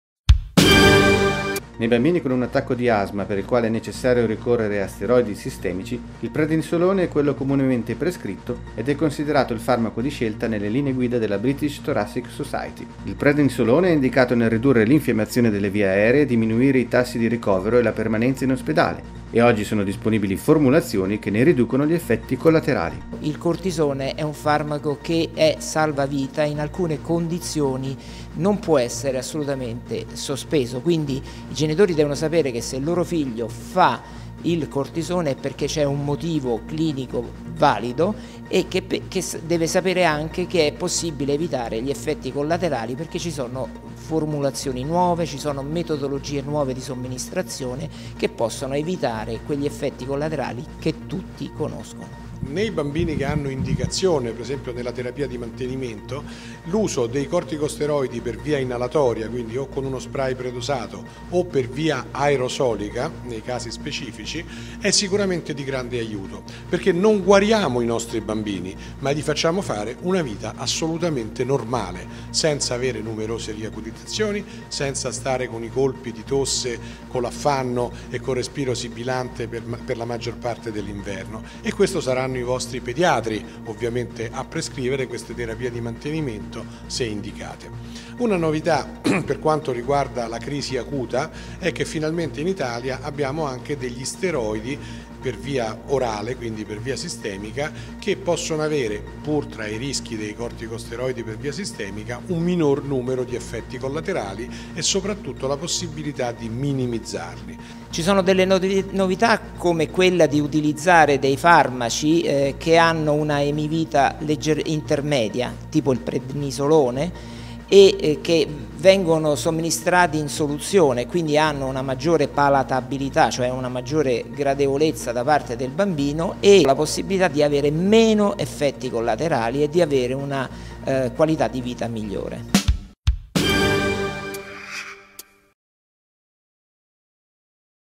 Asma, disponibili corticosteroidi con minori effetti collaterali
Indicati nei bambini che hanno indicazione per questa terapia

Nel video:

Marco CAPPA

Dipartimento di Pediatria Ospedale Pediatrico Bambino Gesù Roma

Renato CUTRERA

Presidente Società Italiana Malattie Respiratorie Infantili